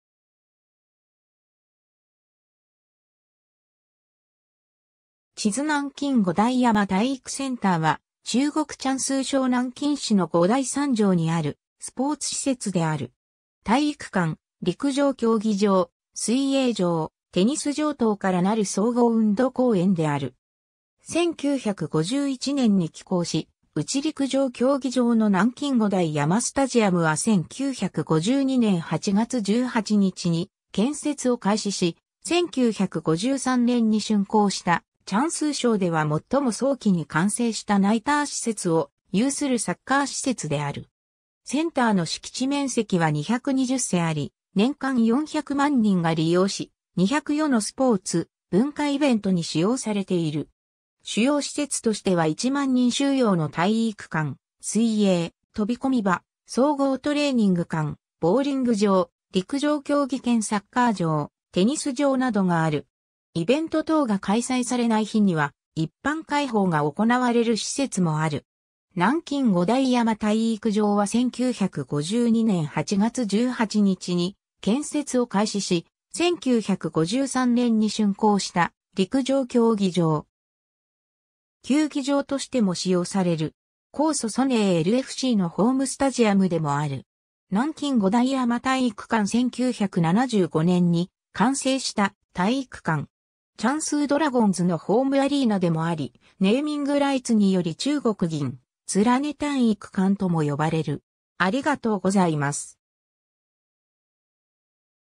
南京五台山体育センター
鼓楼区_(南京市)
地図 南京五台山体育センター（なんきんごだいさんたいいくセンター、簡体字:南京五台山体育中心）は中国江蘇省南京市の五台山上にあるスポーツ施設である。
体育館、陸上競技場、水泳場、テニス場等からなる総合運動公園である。
 1951年に起工し、うち陸上競技場の南京五台山スタジアム（南京五台山体育场）は1952年8月18日に建設を開始し、1953年に竣工した、江蘇省では最も早期に完成したナイター施設を有するサッカー施設である。
センターの敷地面積は220畝あり、年間400万人が利用し、200余のスポーツ、文化イベントに使用されている。
 主要施設としては1万人収容の体育館、水泳、飛び込み場、総合トレーニング館、ボウリング場、陸上競技兼サッカー場、テニス場などがある。
イベント等が開催されない日には一般開放が行われる施設もある。
 南京五台山体育場（南京五台山体育场）は1952年8月18日に建設を開始し、1953年に竣工した陸上競技場。
江蘇蘇寧LFCのホームスタジアムでもある。
 南京五台山体育館（南京五台山体育馆）1975年に完成した体育館。
江蘇ドラゴンズのホームアリーナでもあり、ネーミングライツにより中国銀聯体育館とも呼ばれる。